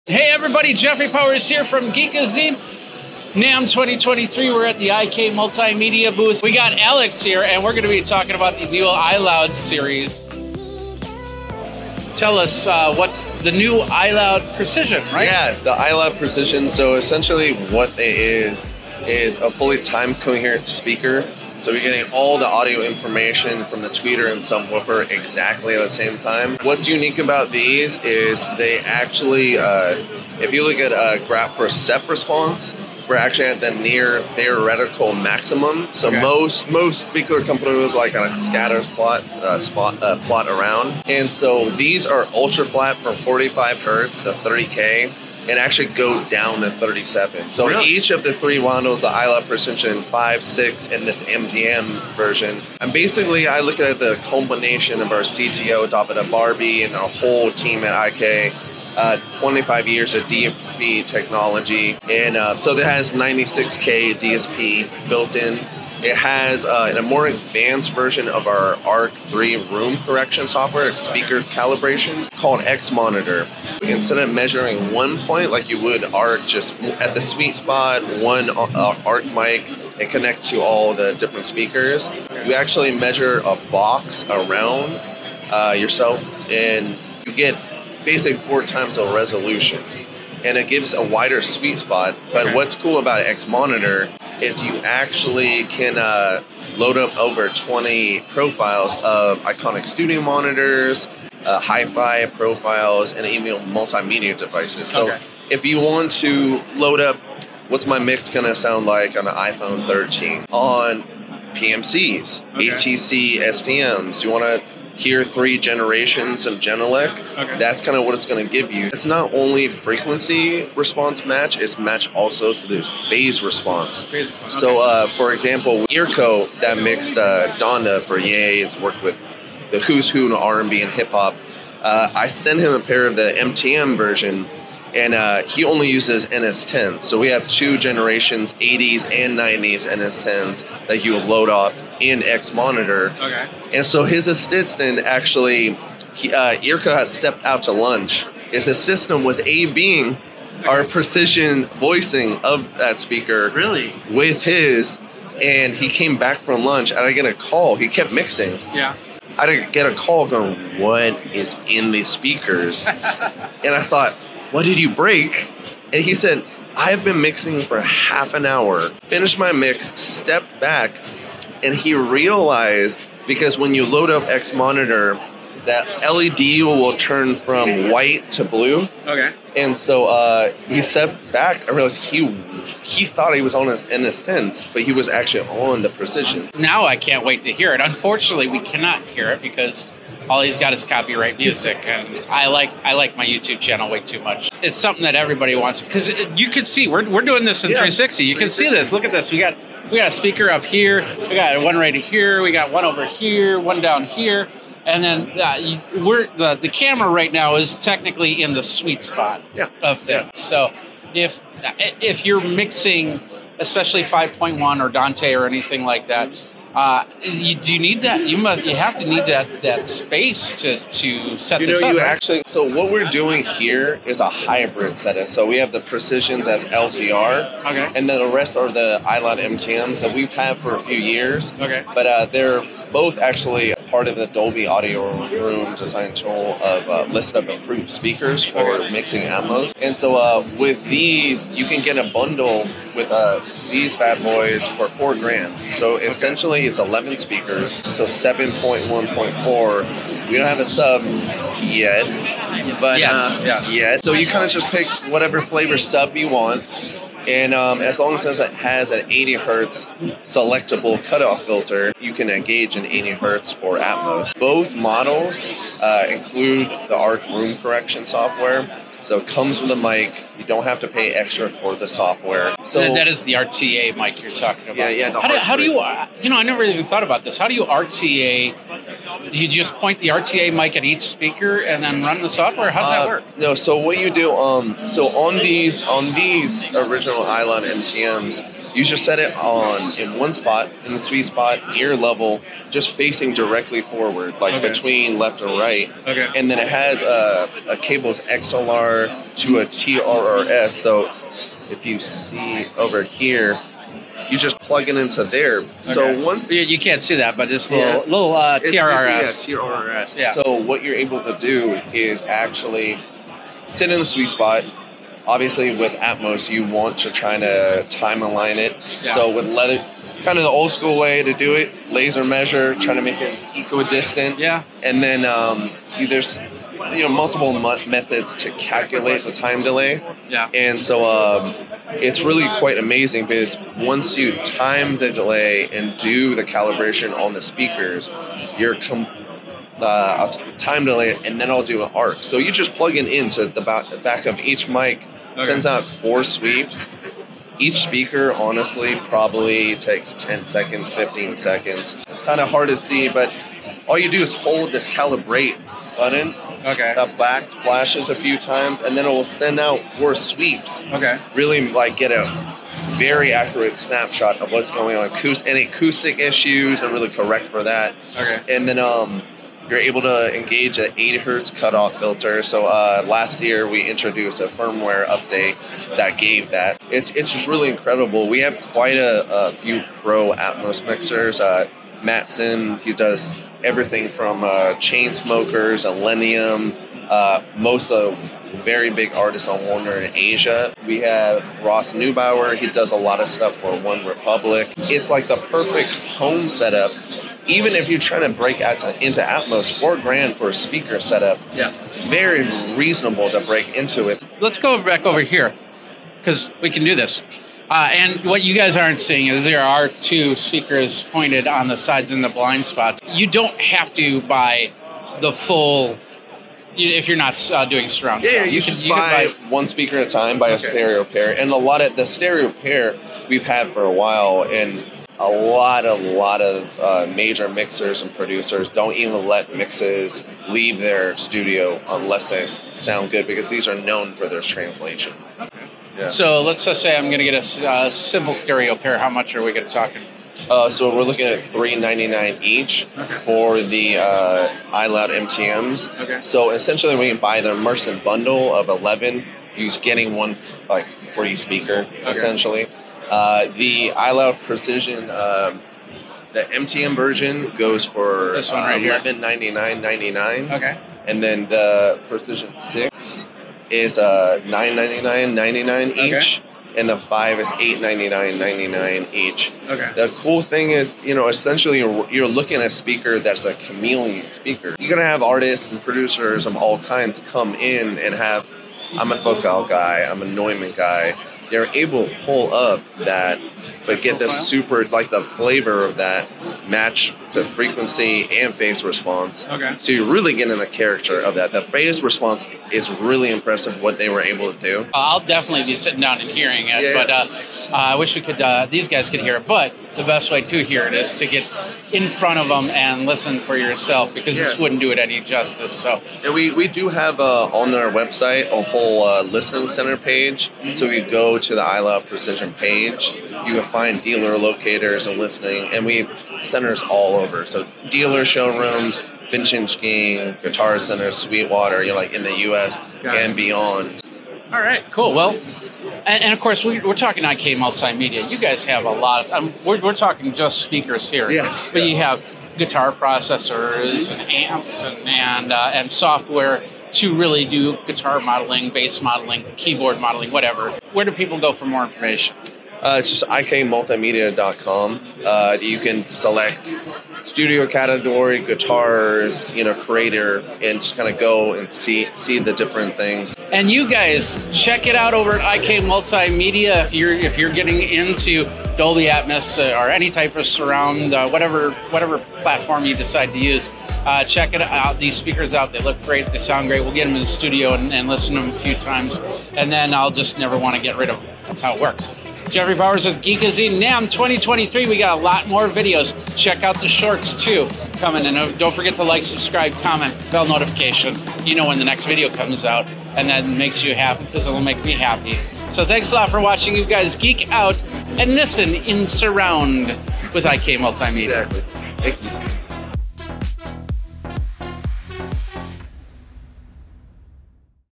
Advanced Audio Production with iLoud Precision Monitor Speakers
I talk with Alex from @ikmultimedia about the iLoud Precision Monitor Speaker system at @nammshowchannel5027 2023, and hear some great stories on how this system helps shapes the music we listen

Do you use iLoud Precision Monitor Speakers? Let me know!